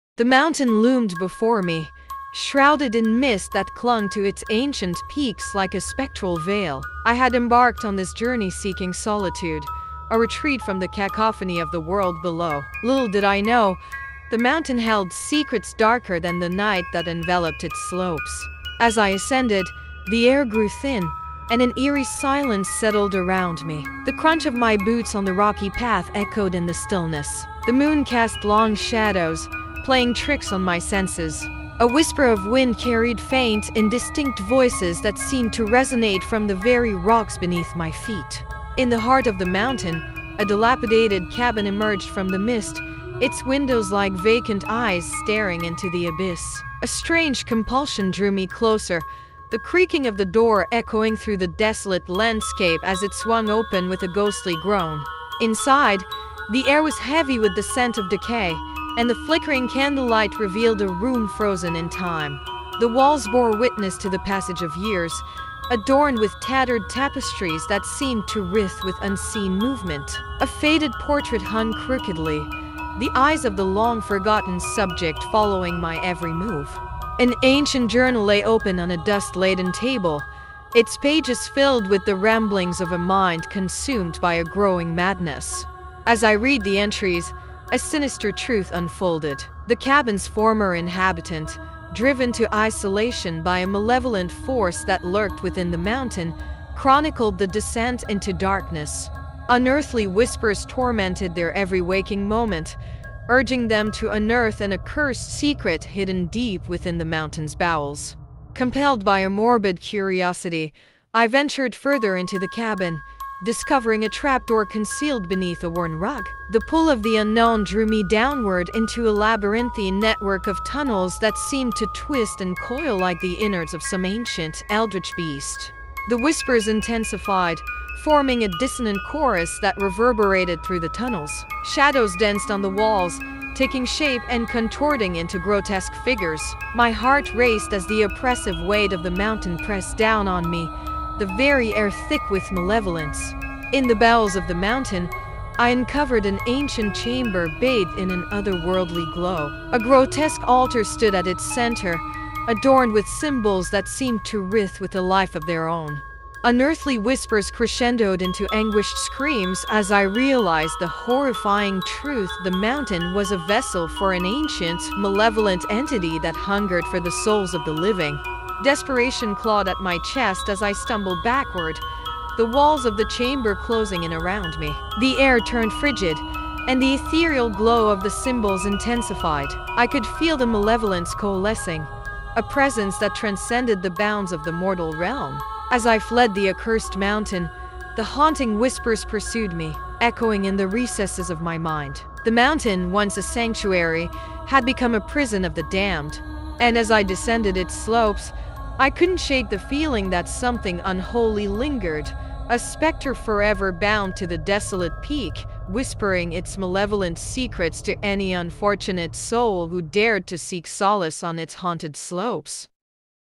The Whispering Peak: A Descent into Madness
"Embark on a chilling journey into the heart of 'The Whispering Peak: A Descent into Madness,' where an isolated mountain conceals ancient horrors and unearthly secrets, driving those who seek solace to the brink of insanity."